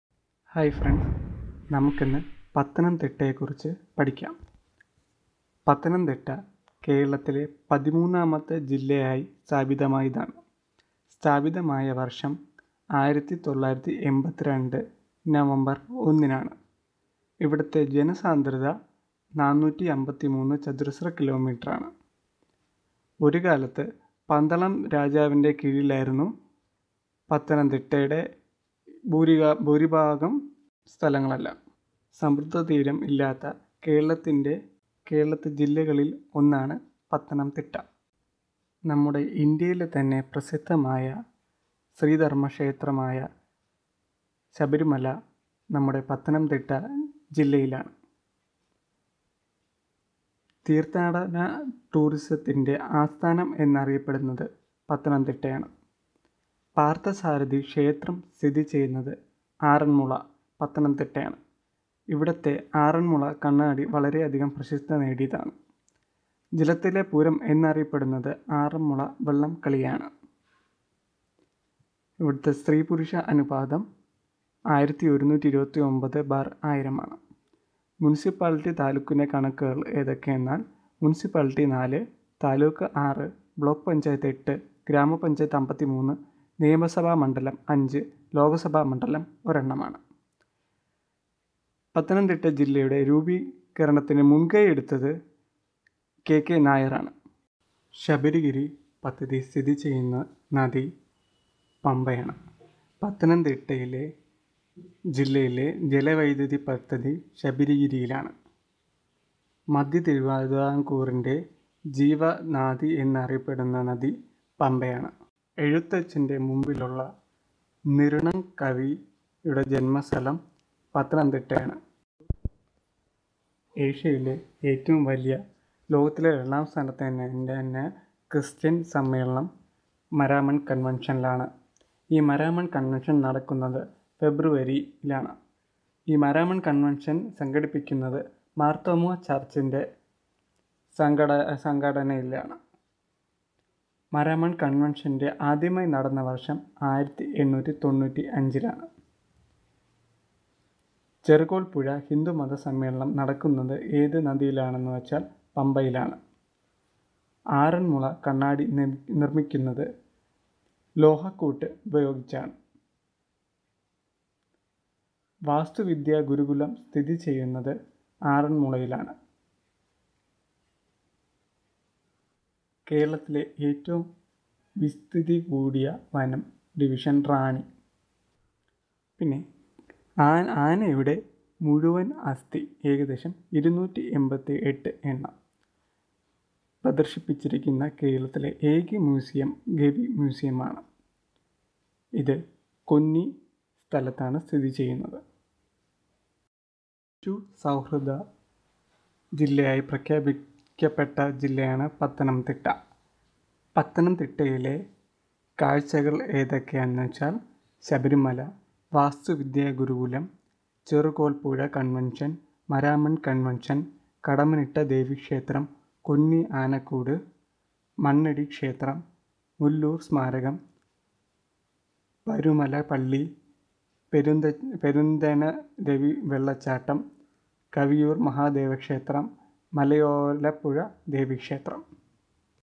പത്തനംതിട്ടജില്ലയുടെ അടിസ്ഥാന വിവരങ്ങൾ |About Pathanamthita PSC| കേരള പി എസ് സി സ്പെഷ്യൽ
Pathanamthita Basic information includes population, village, large and small relations in different questions,etc.
This Kerala basic information consists of Kerala PSC exam topics.